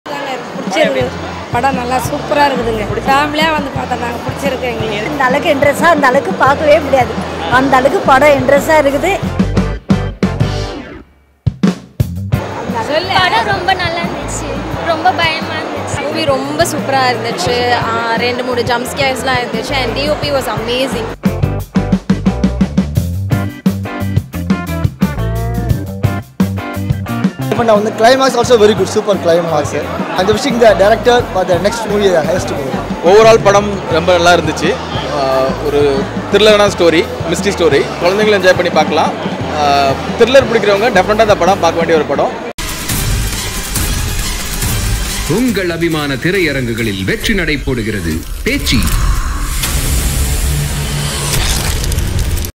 Pechi Parents response | Pechi movie | Bala saravanan | Gayathri | Verus Productions
This video is a combined reviews of movie Pechi